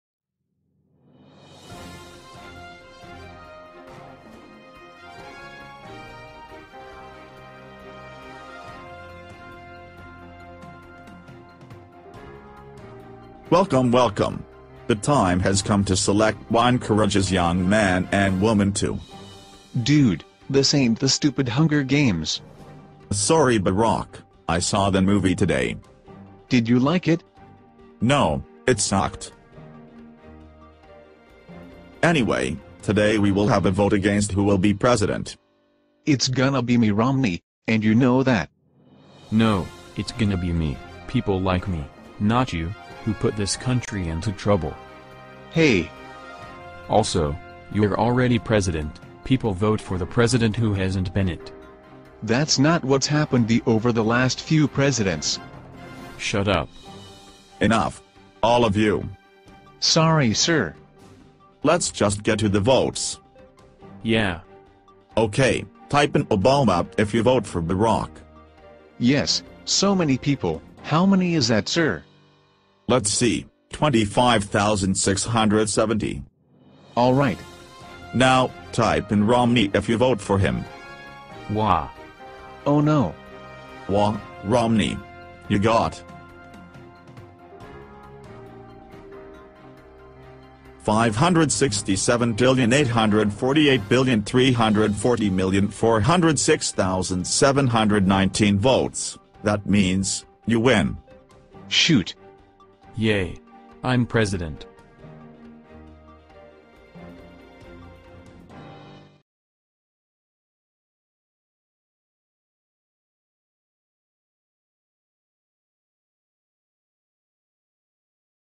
ROMNEY VS. OBAMA
Barack and Mitt have a fight against who will be president!